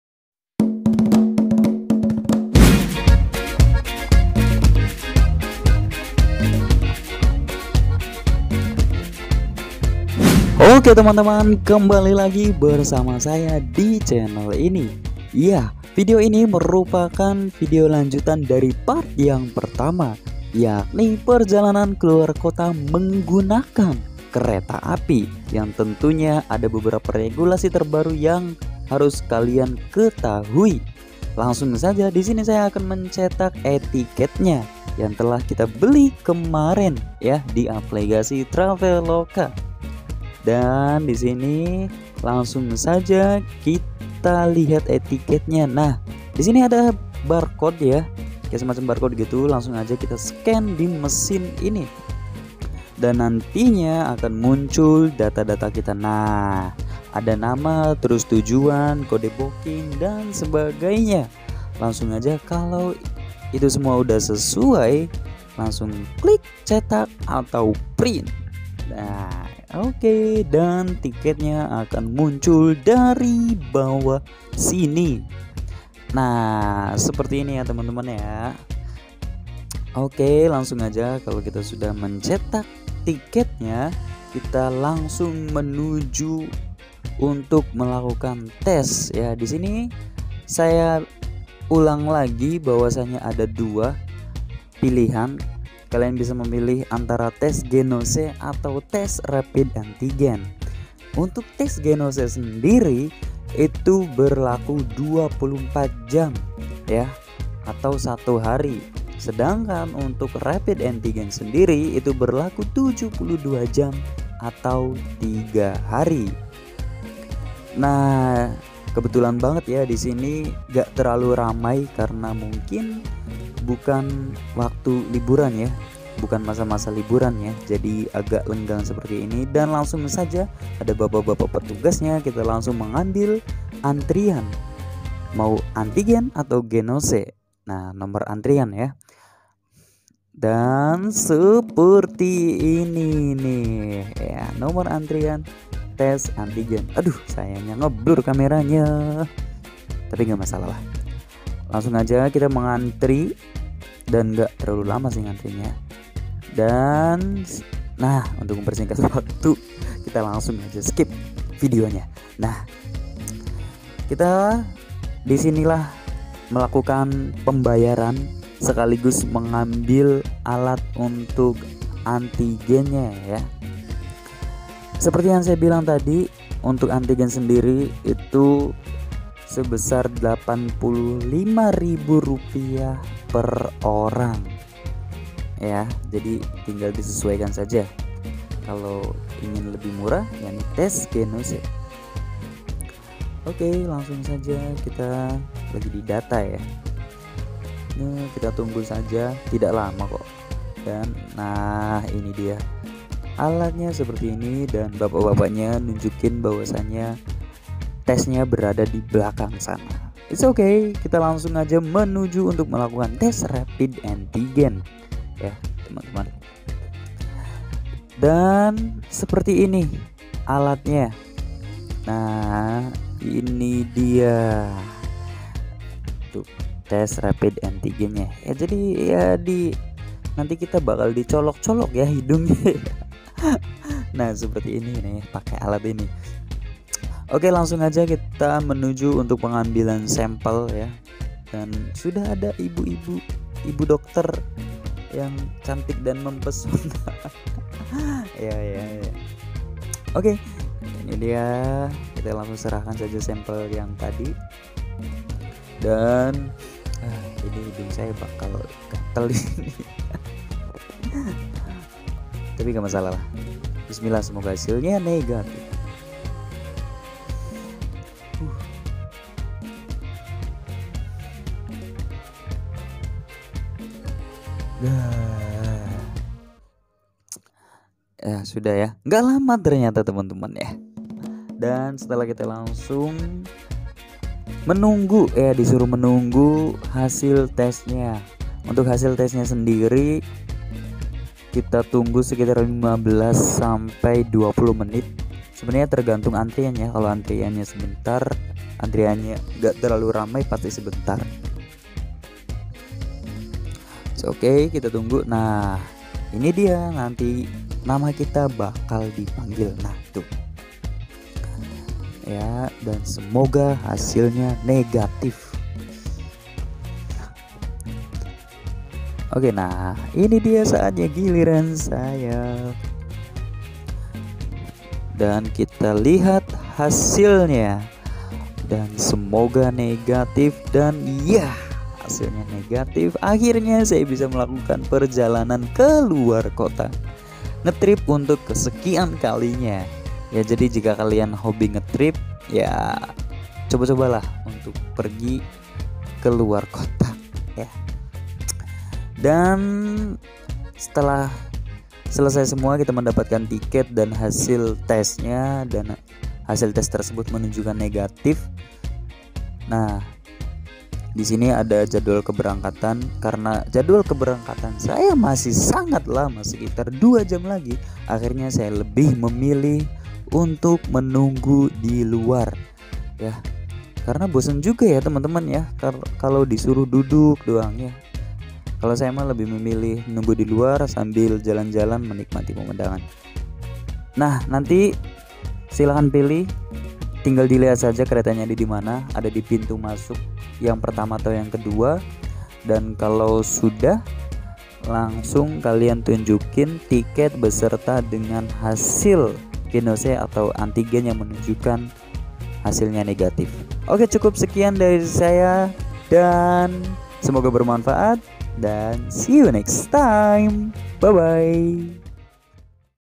Cara naik kereta api di masa pandemi - (part 2: Tes antigen/Genose di stasiun)
Cara naik kereta api di masa pandemi memang sangatlah berbeda karena ada beberapa syarat untuk naik kereta yang harus kalian ketahui pada saat naik kereta di era new normal ini. syarat tersebut bermacam macam tapi yang paling penting adalah ketika ingin naik kereta di masa pandemi kalian diwajibkan untuk melakukan tes rapid antigen atau genose sebelum keberangkatan. untuk tes antigen/genose di stasiun sekarang sudah tersedia walaupun tidak semua stasiun menyediakan fasilitas tersebut. lantas bagaimana cara tes antigen/genose di stasiun? berapa harga yang harus dibayar untuk satu kali tes? dan regulasi apa yang berlaku di stasiun? untuk informasi lebih lengkap nya silahkan tonton videnya sampai habis...